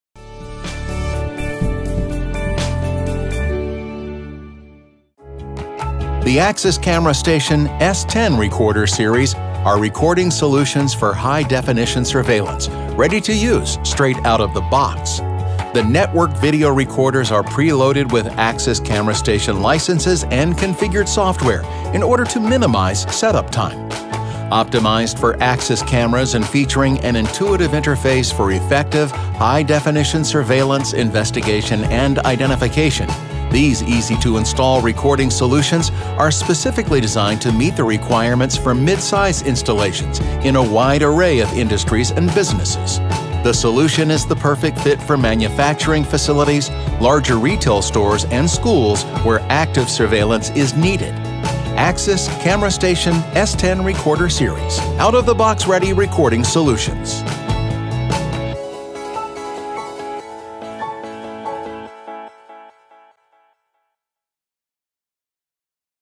Axis - Camera Station S10 Recorder Series
The Axis Camera Station S10 Recorder Series are recording solutions for high-definition surveillance.